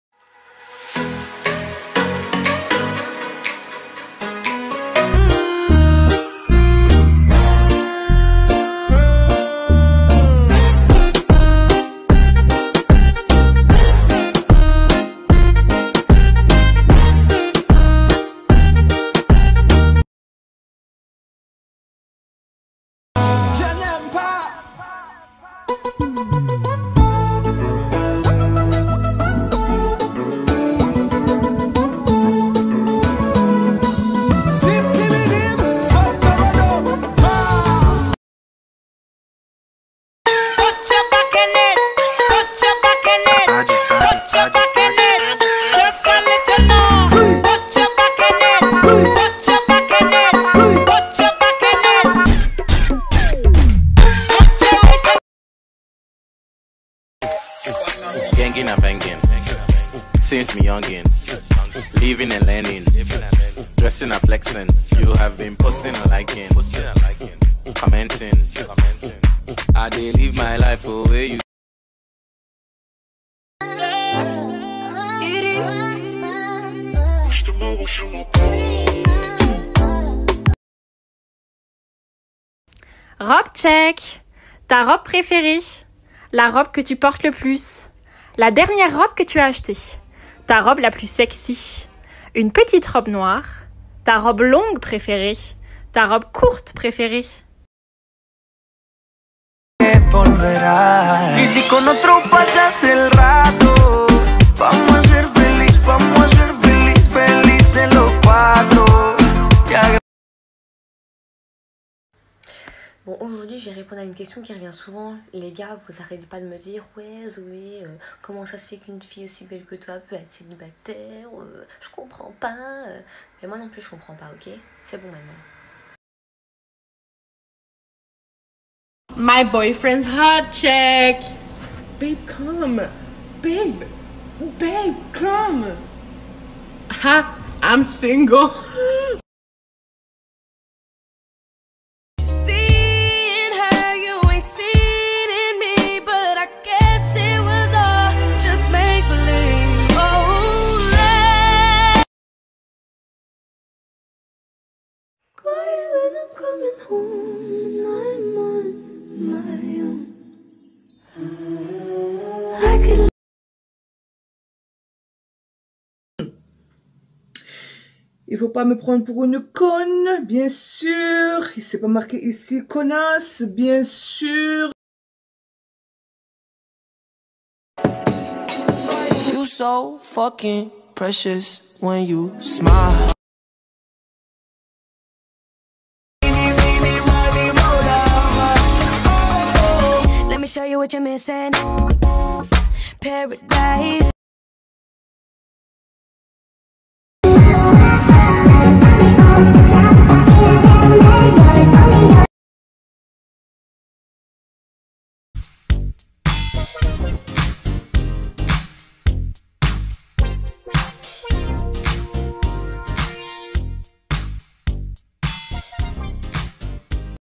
Zoé Brunet tiktok
Zoé Brunet is a Belgian model and beauty pageant tittleholder who placed first runner- up at Miss Belgium 2018, Representimg Namur. She was later appointed Miss Universe Belgium 2018, and represented Belgium at the Miss universe 2018.

Follow Zoé Brunet!